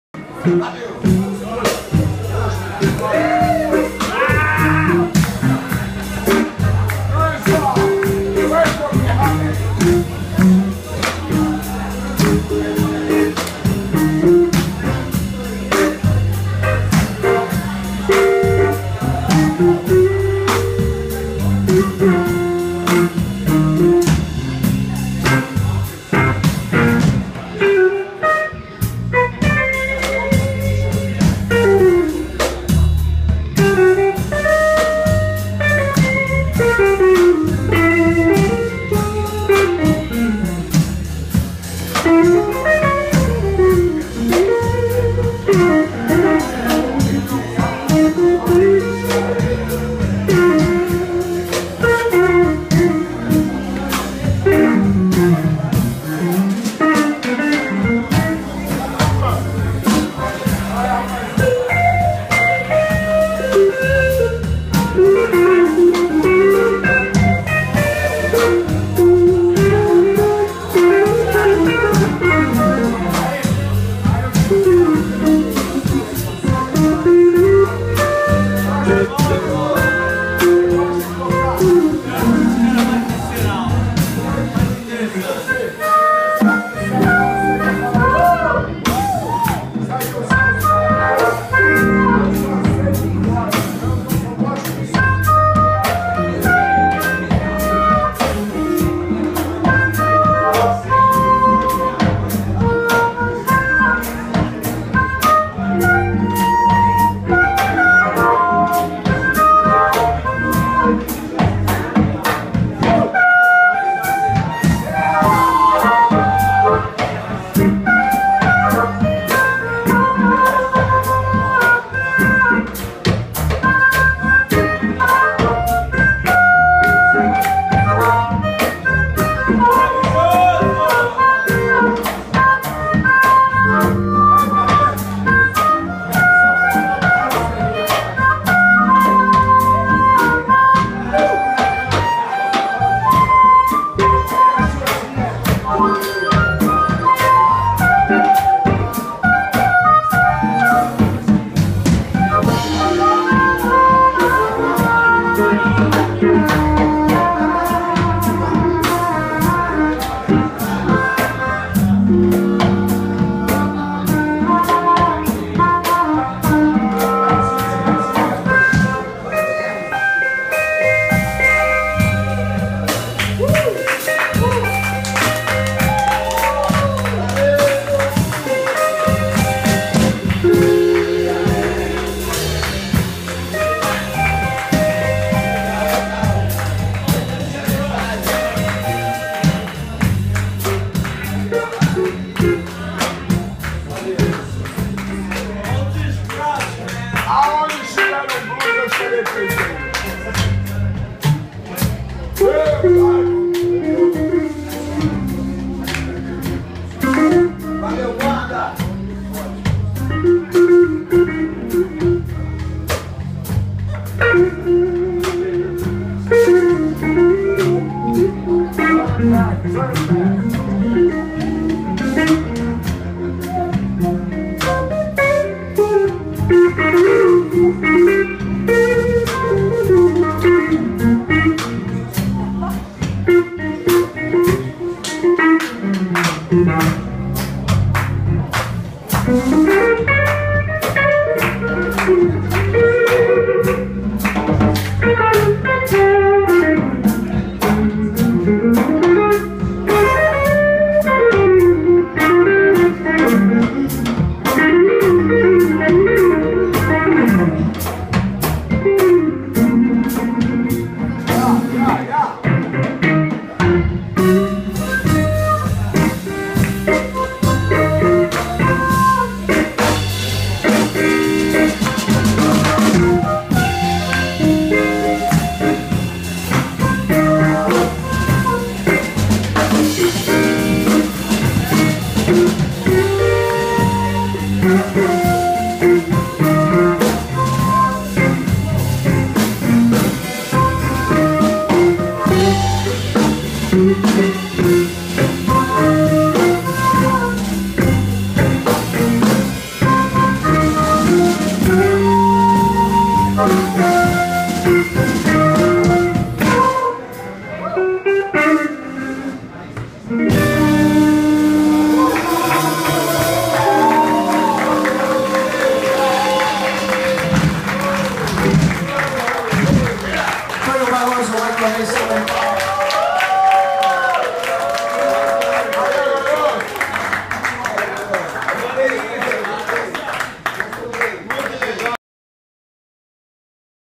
Artur Menezes / Toyo - Bar do Panq´s Pub - RJ - 08/01/2016
Artur Menezes - Guitarra e Vocal
Toyo - Harmonica
Cesar Lago - Baixo
Beto Werther - Bateria